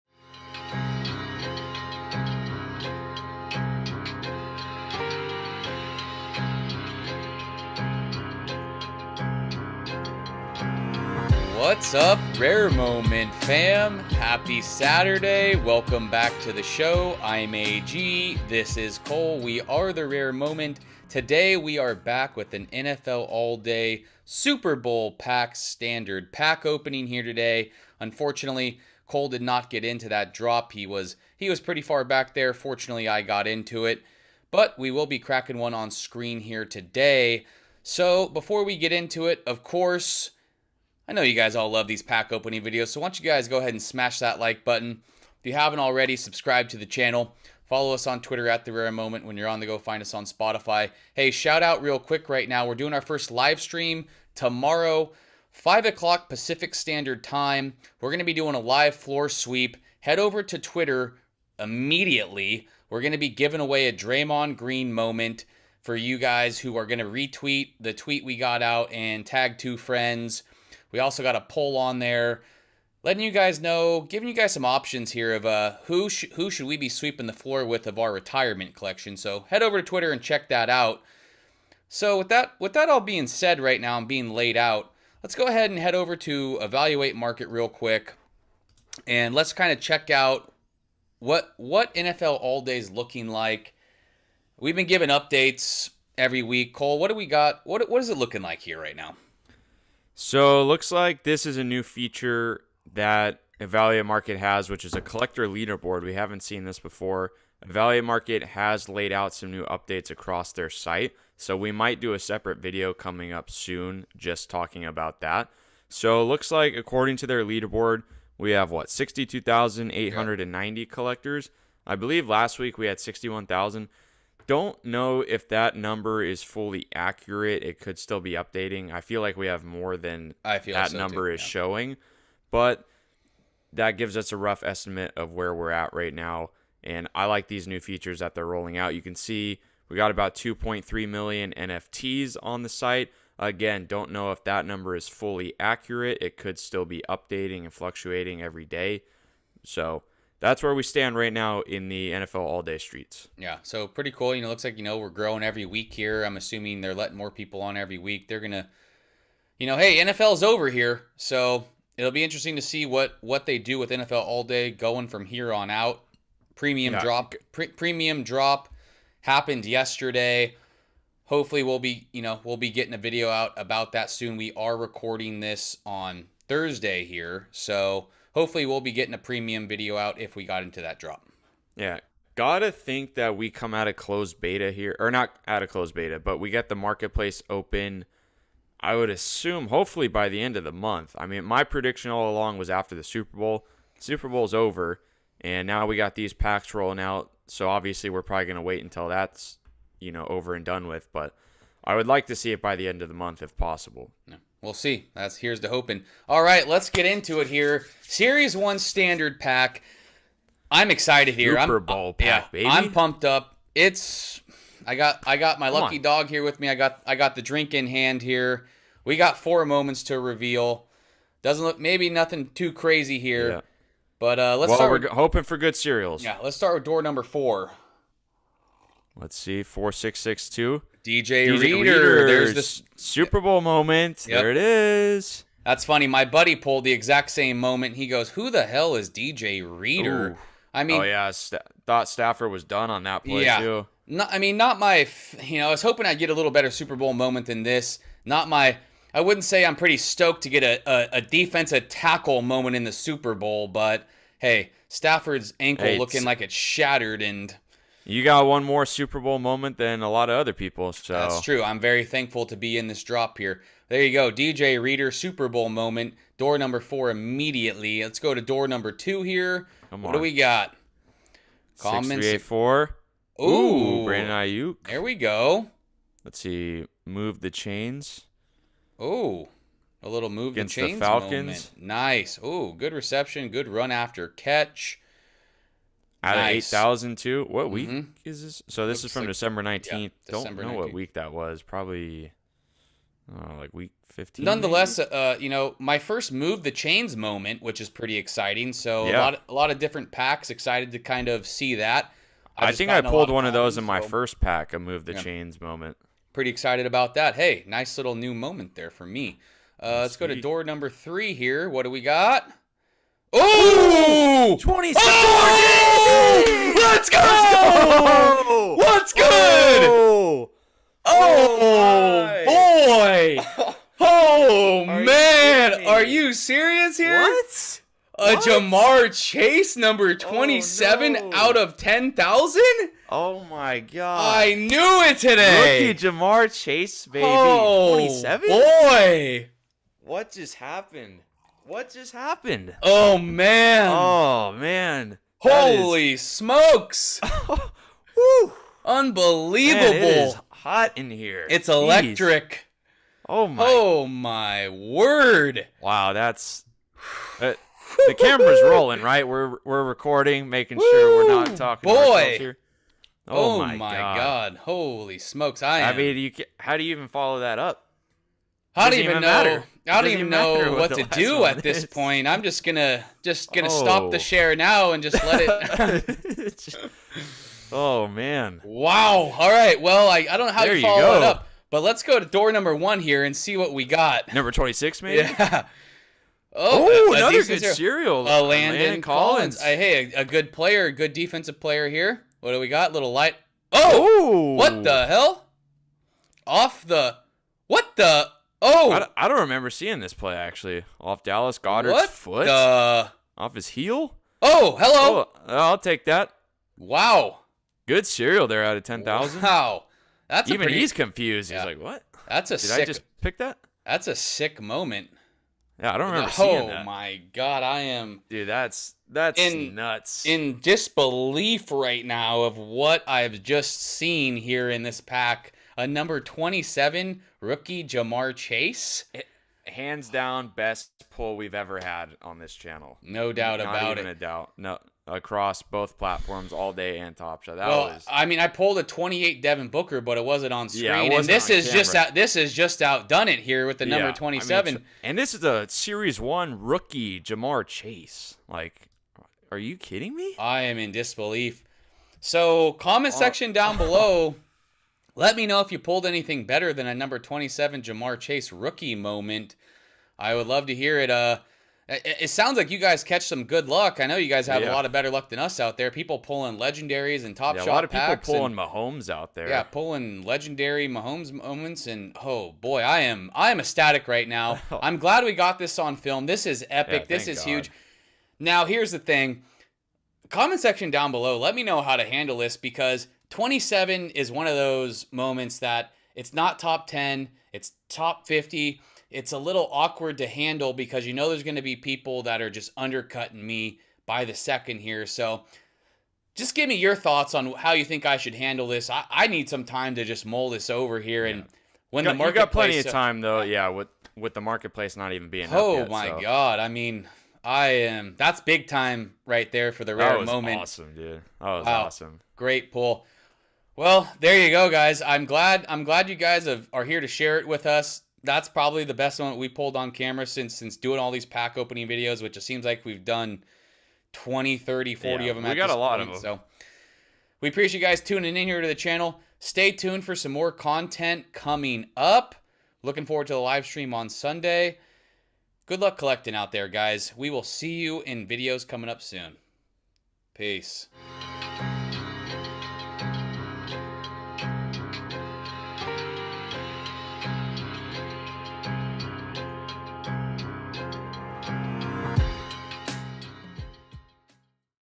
NFL ALL DAY Standard Super Bowl 56 Pack Opening - UNREAL Pull!!
Welcome to The Rare Moment!

AG's ripping open a Standard Super Bowl 56 pack! We still can't believe what he pulled!!

AG's Top Shot/ALL DAY Username: Pianoman26
Cole's Top Shot/ALL DAY Username: ProfessorAce